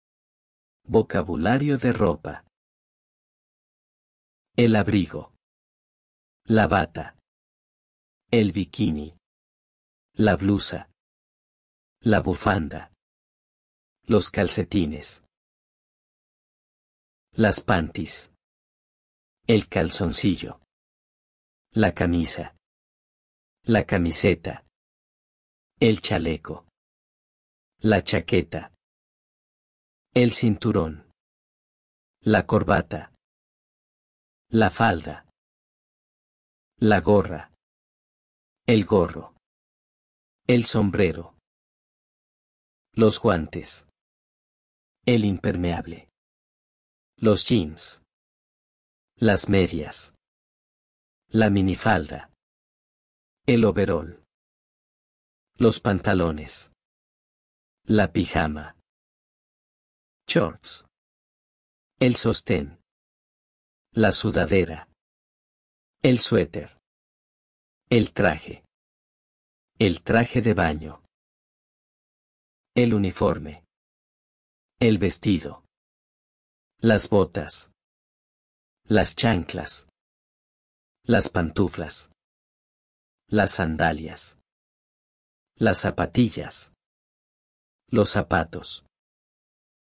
Clothing vocabulary/Vocabulario de la ropa🩱👗👔
Do you know the words for your clothes in Spanish? If not, in today's video, you will learn the vocabulary for clothing in Spanish. If you want to learn it, click below to study these words in flashcards👇